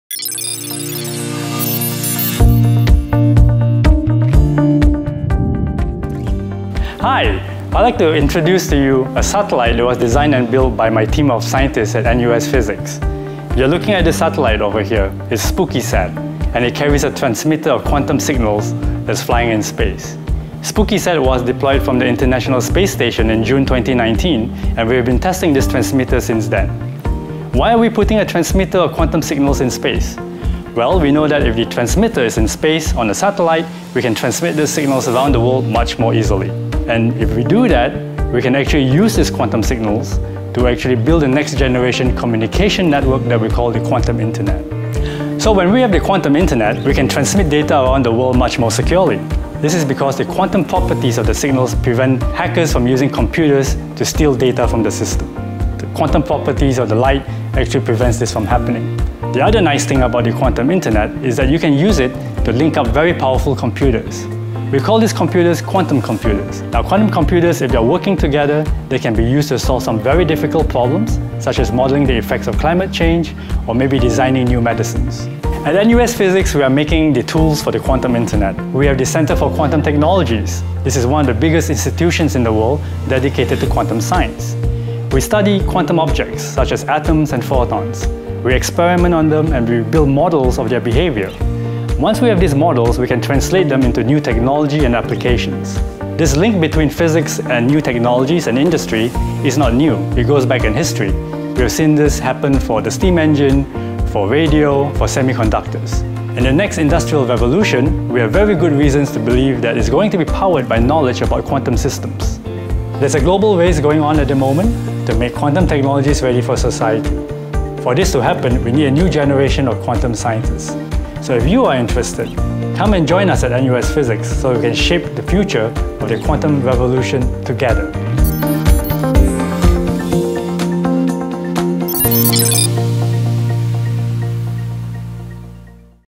Surprising Science! ~ A Quantum Leap into the Future
Prof Ling introduces SpooQy-1, the world’s first compact quantum nanosatellite which provides a glimpse into innovative  satellite communication technology of the future. He also shares how physicists at NUS are building quantum satellites and other quantum devices for next generation communications.

Prof Alexander Ling
NUS Department of Physics